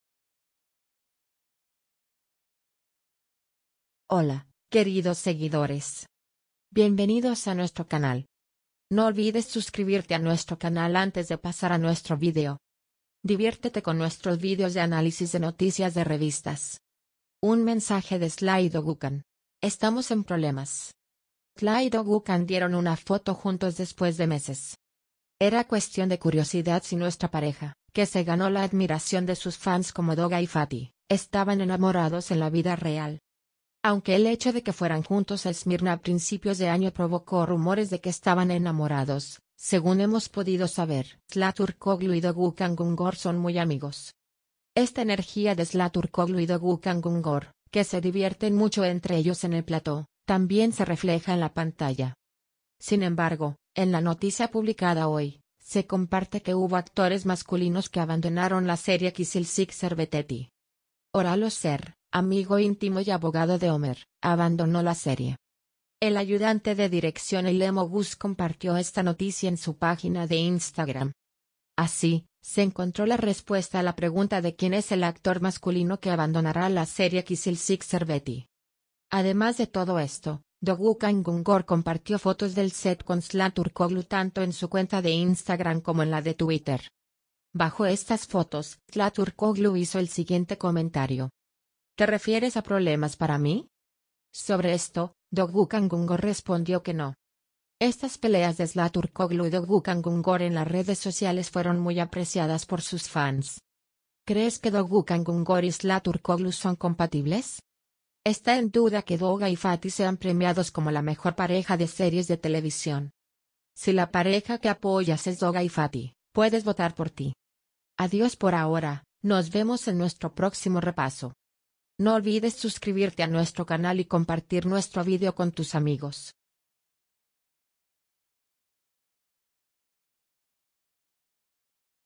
Un mensaje de Sıla y Doğukan. Estamos en problemas.
Sıla y Doğukan dieron una foto juntos después de meses.
Era cuestión de curiosidad si nuestra pareja, que se ganó la admiración de sus fans como Doğa y Fatih, estaban enamorados en la vida real.
Aun